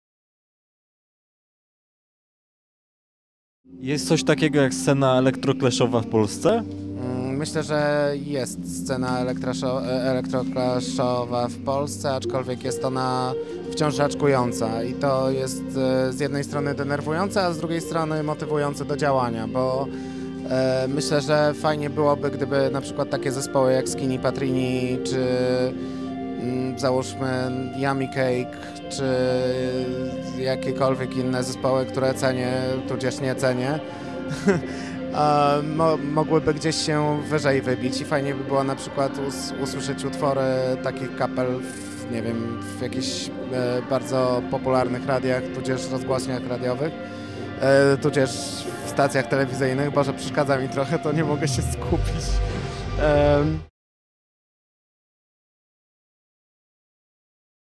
Skinny Patrini wywiad dla cgm.pl
O scenie electroclashowej i działalności duetu Skinny Patrini, opowiada jego męska połowa - Michał "Skinny" Skórka.